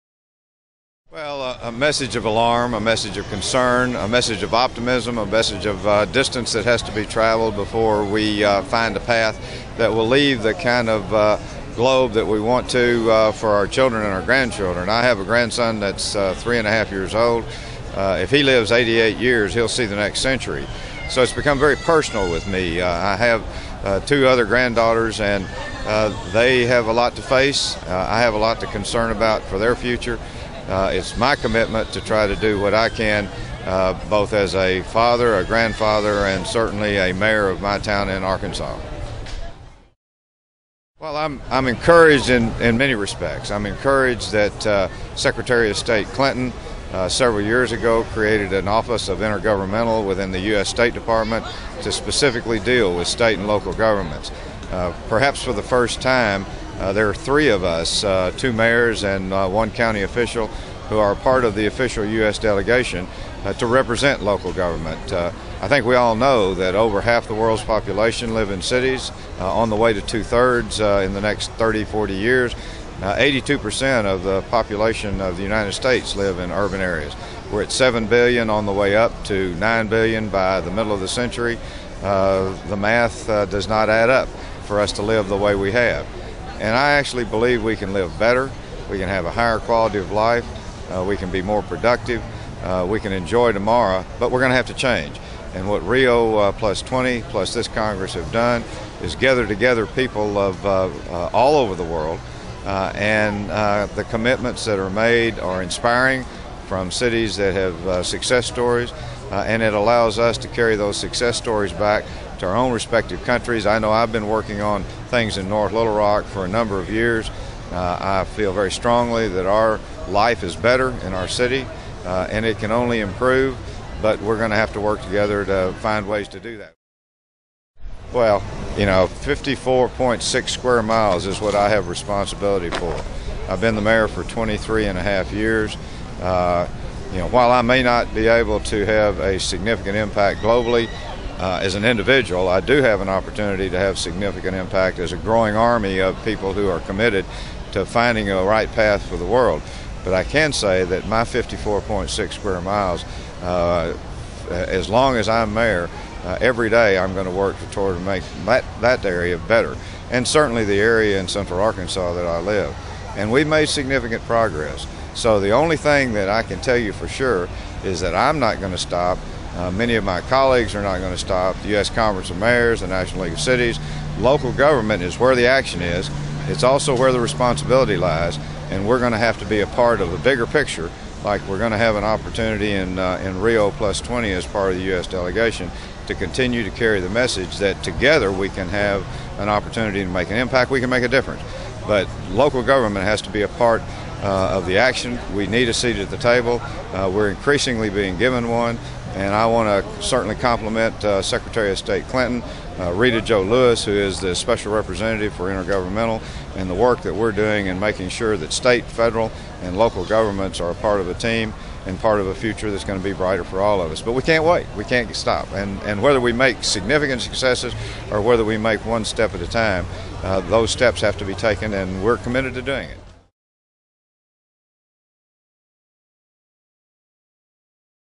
Patrick Hays, Mayor of North Little Rock sends Rio+20 message
Patrick Henry Hays, Mayor of North Little Rock, USA, says that sustainability brings economic progress and a better quality of life to citizens. Moreover local governments need to be at the table of negotiations, when discussing a sustainable future for our planet.